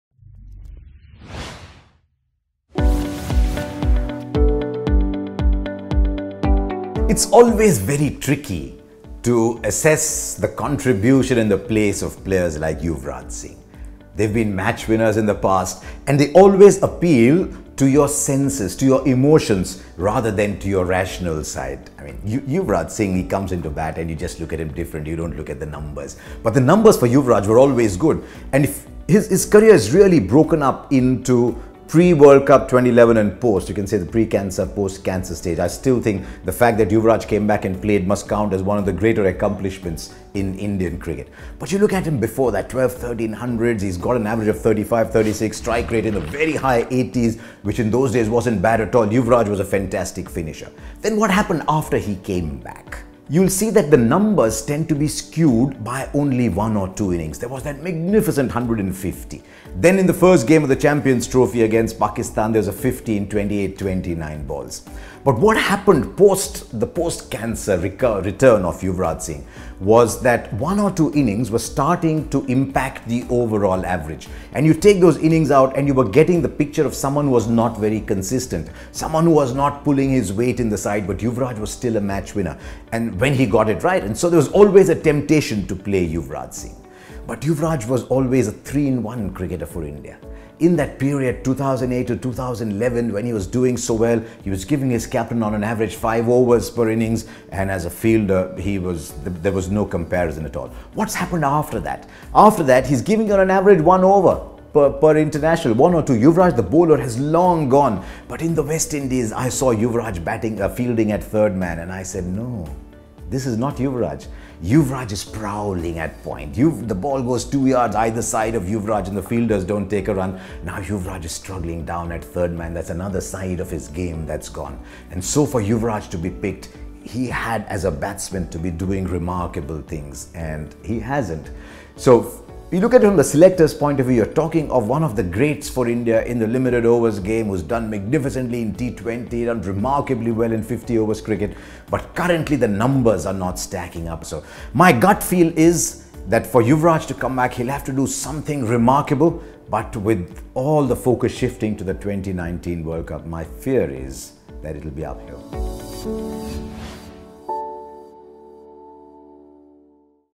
End of the road for Yuvraj Singh?
He was India's hero in the 2007 World T20 & the 2011 World Cup, but Yuvraj Singh now finds himself out of the Indian side after being dropped. Harsha Bhogle looks at whether it could be the end of the road for Yuvi in international cricket.